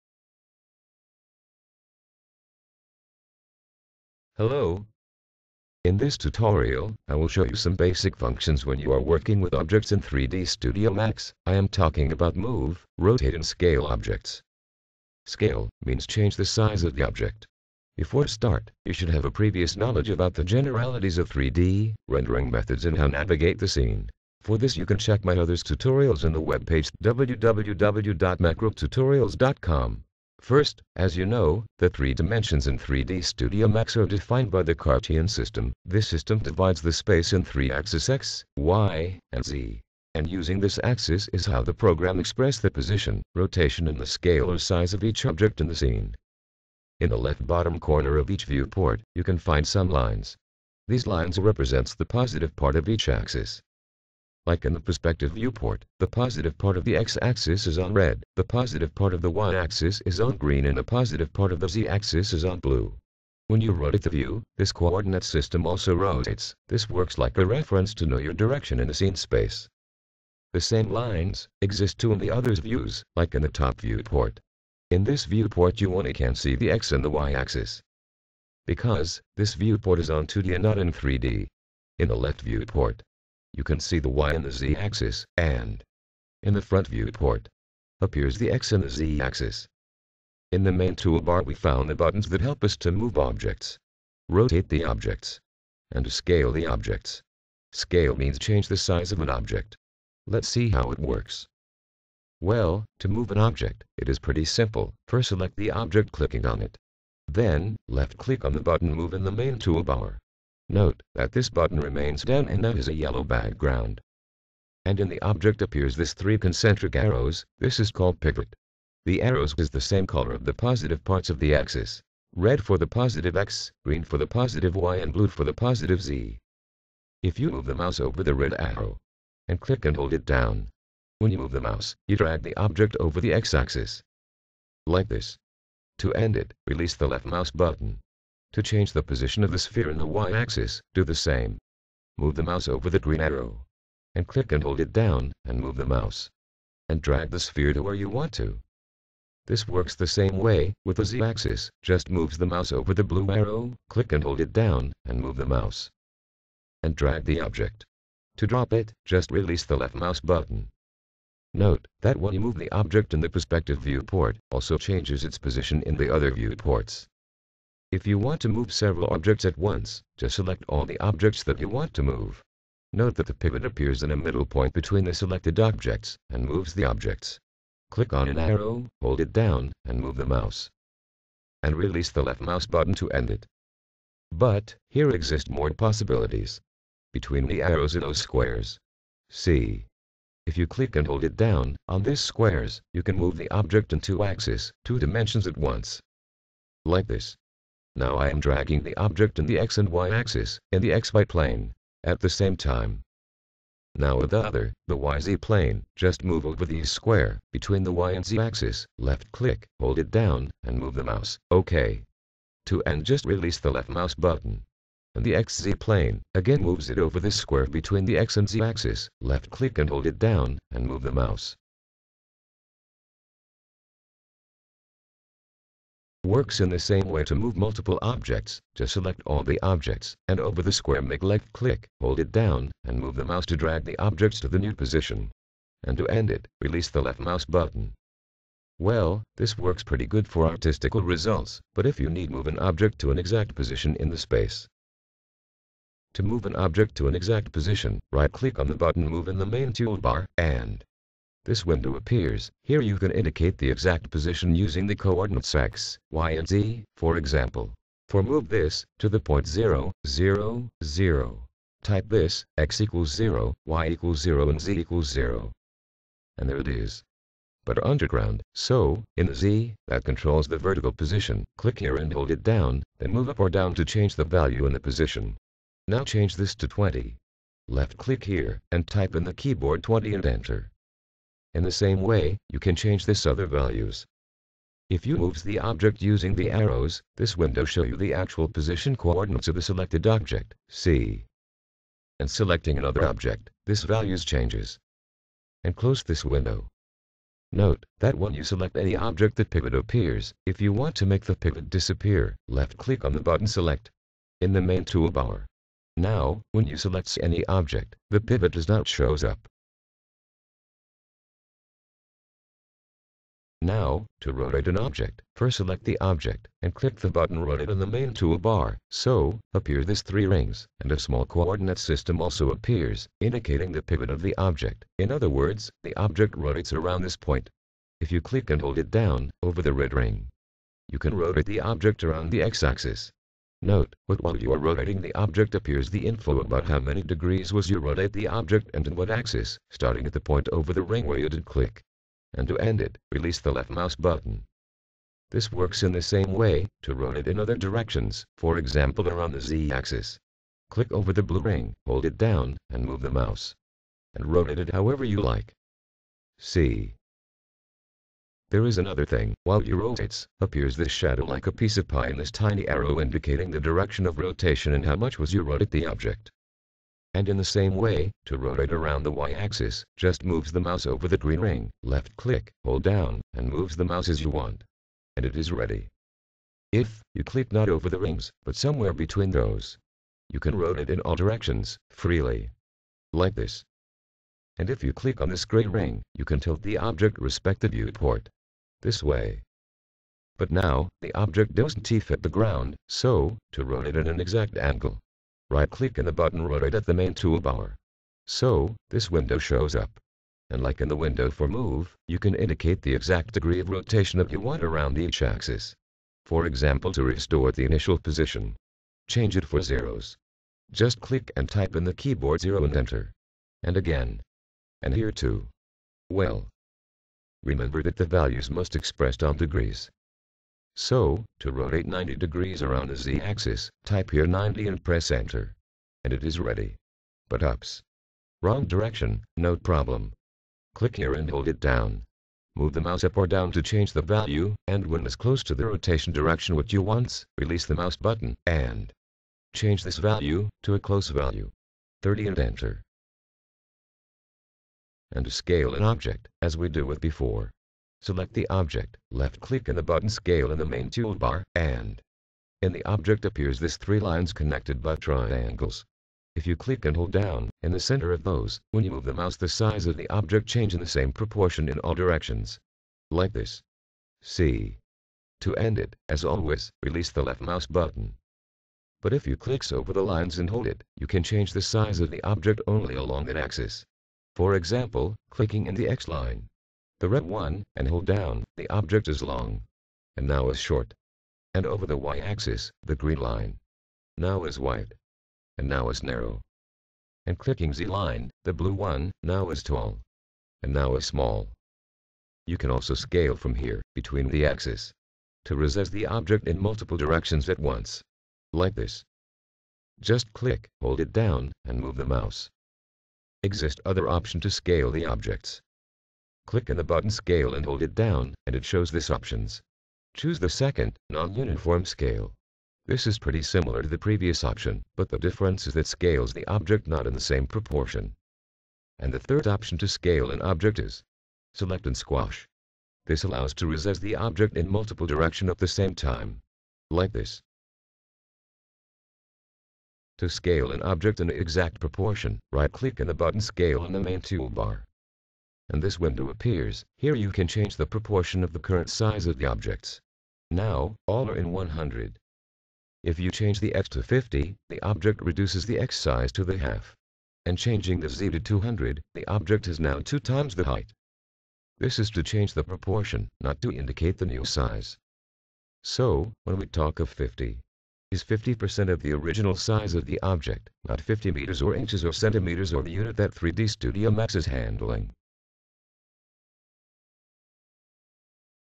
3D Studio Max - Tutorial - How to move, rotate and scale (change the size) the objects
In this tutorial, I will show you some basic functions when  you are working with objects in 3d studio max, I am talking about move, rotate and scale objects.  Scale, means change the size of the object.

CONTENT:

How to move, rotate, scale the objects.

- Cartesian system (029)

- How to move objects and what is the pivot(1:41)

- How to rotate objects (5:12)

- How to scale (change the size) objects (7:24)